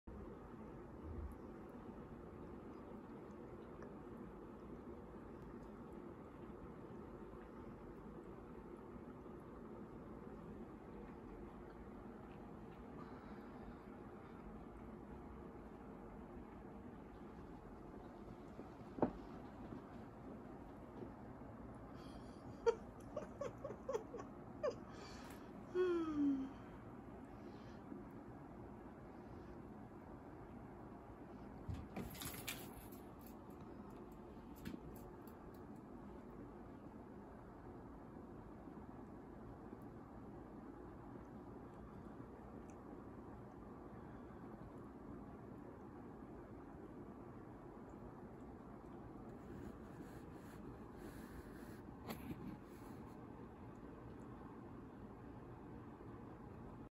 Mr. Noodles Likes the Space Heater
Then Christopher interrupts him as usual.